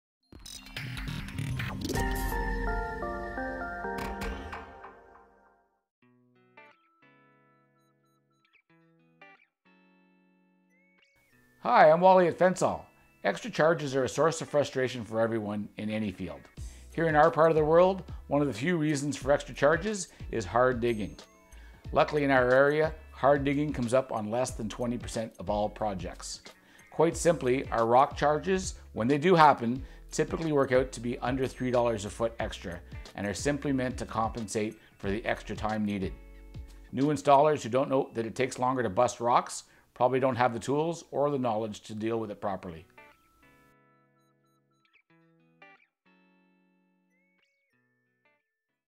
I'm charged extra if there's rock?
There's a surcharge to cover the extra labour.